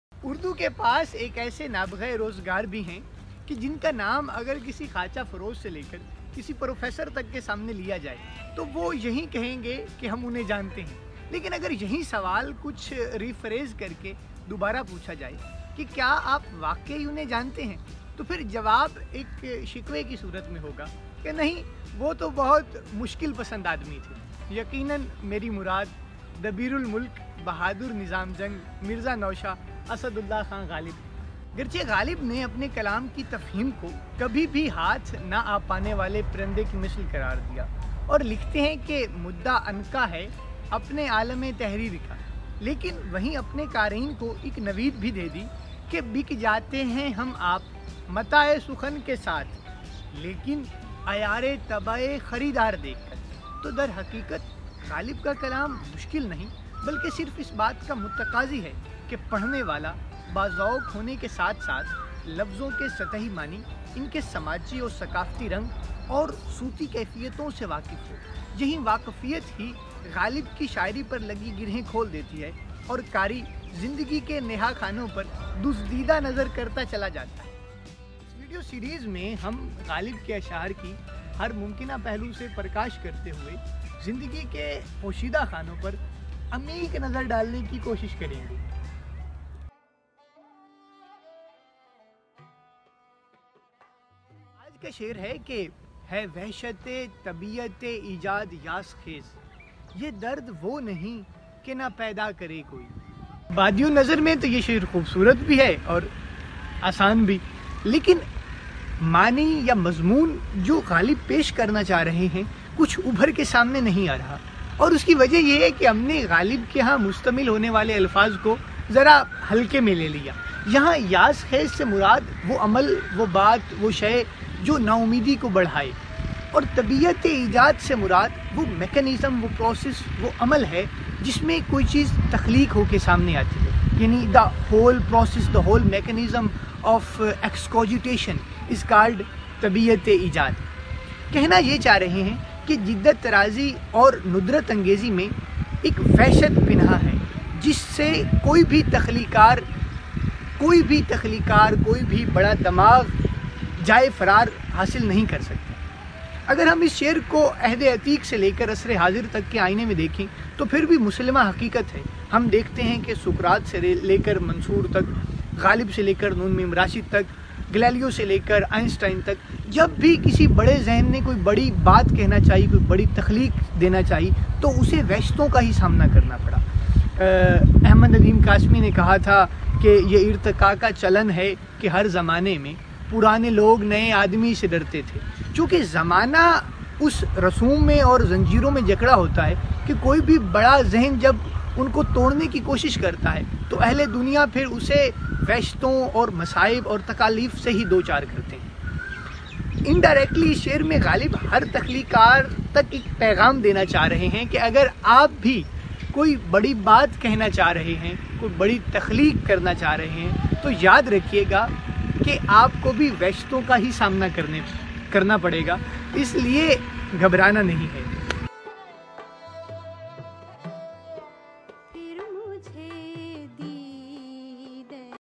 Ghalib Fahmi | غالب فہمی | Decoding Mirza Ghalib Poetry | Ashar ki Tashreeh | Raza Haider
Mirza Ghalib, a great Urdu poet occupying a place of pride in the world of literature, is also one of the most quotable poets who have shared almost all life situations.

CORRECTION:
Sorry! The correct line is " Mu'da anka ha apny Aalm e Tar'reer ka" (مدعا عنقا یے اپنے عالم تقریر کا).

Directed - Madam Gulnaz
Produced - Mr. Rehman Ansari

mirza ghalib best shayari Decoding Mirza Ghalib Poetry  | Ashar ki Tashreeh